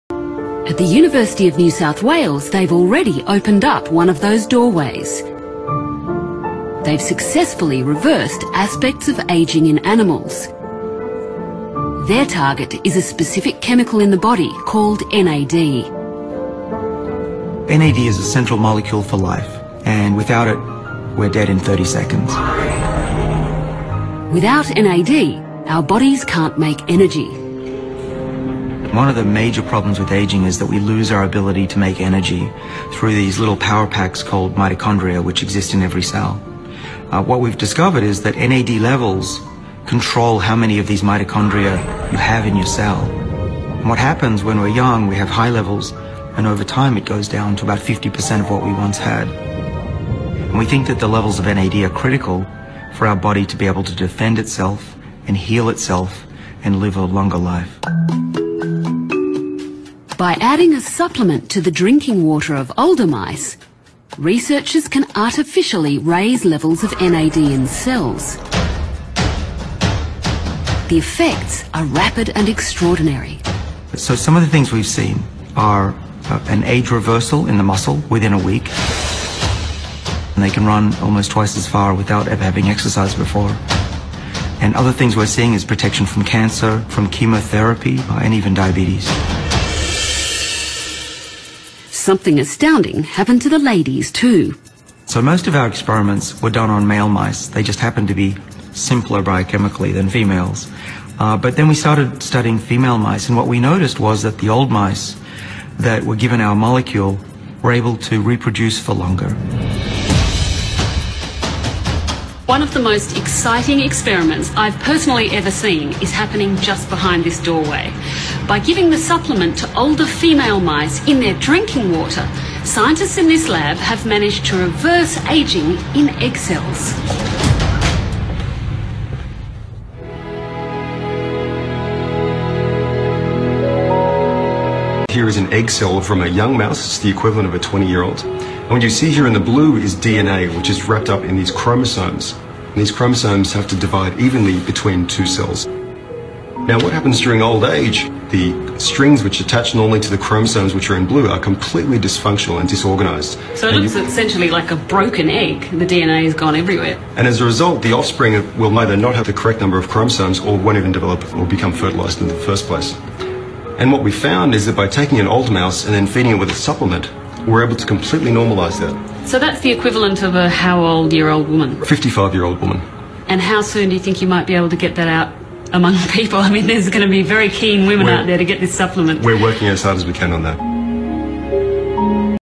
2013年David Sinclair發表NMN研究成果
2013年哈佛醫學院終身教授，發表震驚全球的NMN研究成果。由老鼠週服用兩次NMN後，一週內明顯逆齡回春，運動跑步距離增加2倍。甚至發現預防癌症，放射傷害及糖尿病效果。另外甚至可讓高齡女性卵子 回春，大大延長生育年齡！